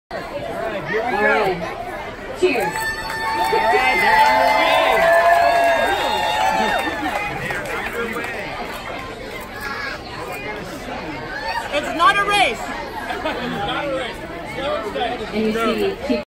"It's not a race" just Chiefs eating a whole bunch of donut holes for a good cause
Grass Valley Police Chief Alex Gammelgard, Nevada County Sheriff Shannan Moon and Nevada City Police Chief Ellis - with some help - consumed some 120 donut holes each. This was their part in a fundraiser to offset the loss experienced by Grass Valley Charter School Foundation after they cancelled their Blue Marble Jubilee fundraising event due to conspiracy theorist threats.